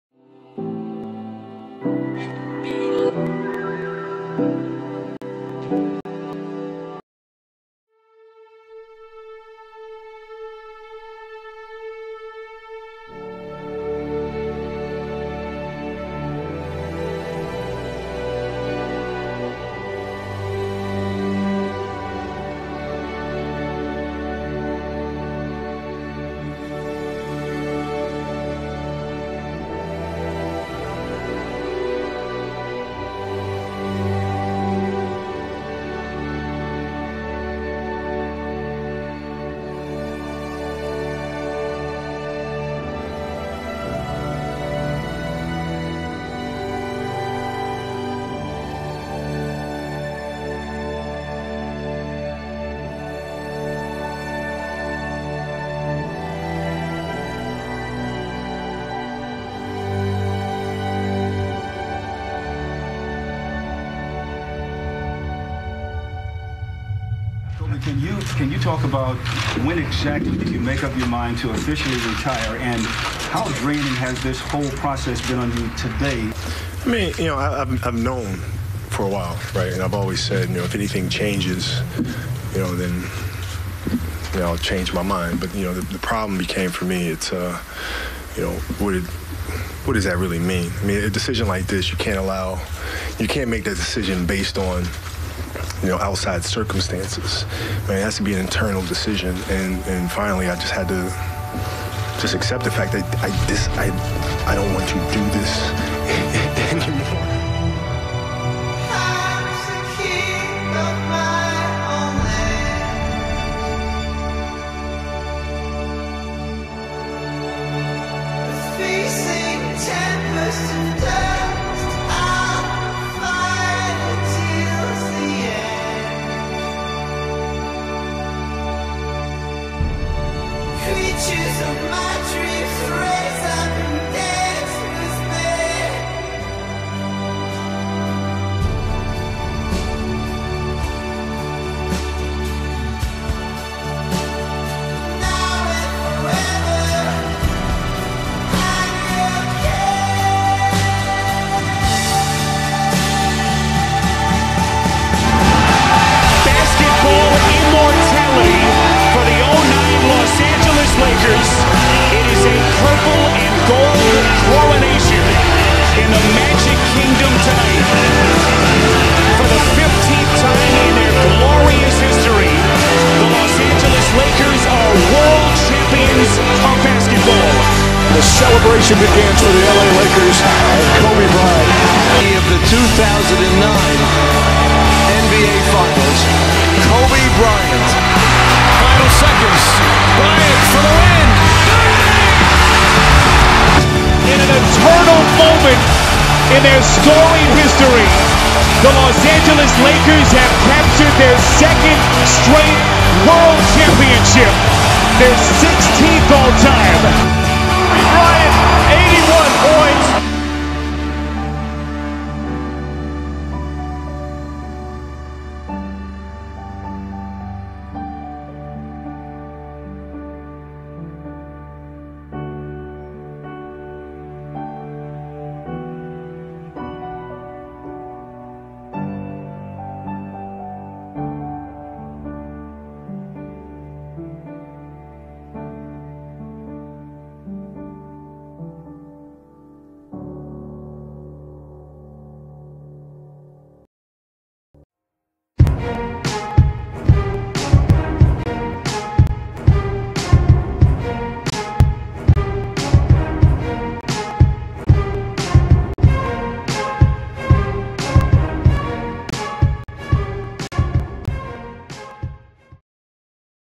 Kobe Bryant - Outro ᴴᴰ
RIP KOBE BEAN BRYANT 💔🙏 🐍

RIP GIGI BRYANT💔🙏

Mix of my favorite Athlete of all time, my inspiration, Kobe Bean Bryant. This was supposed to originally go up on April 13th (Date of Kobe's last game) but my computer had some problems. : (
Computer is fixed so expect many videos during the summer!